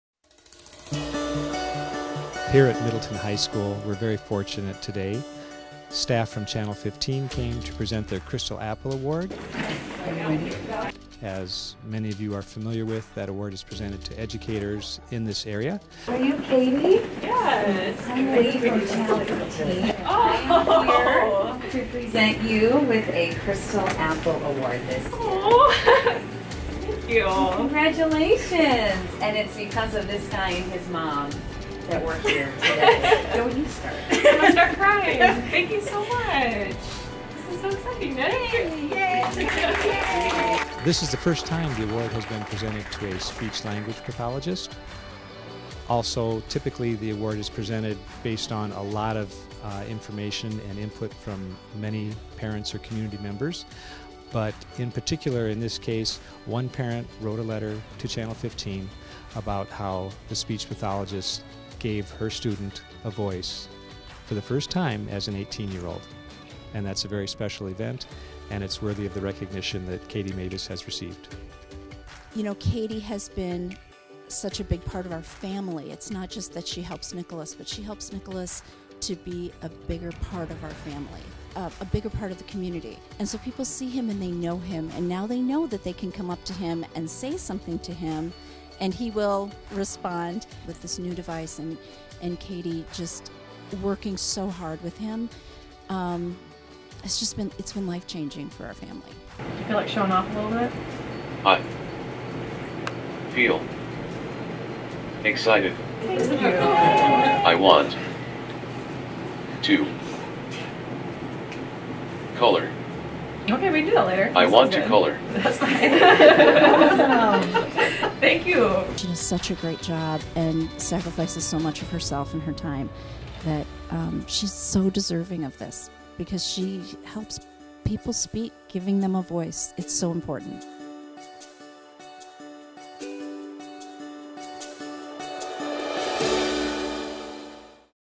Middleton Minute CrystalAppleAward
Middleton Minute- Crystal Apple Award presented to
Middleton High School Speech and Language Katelyn Meves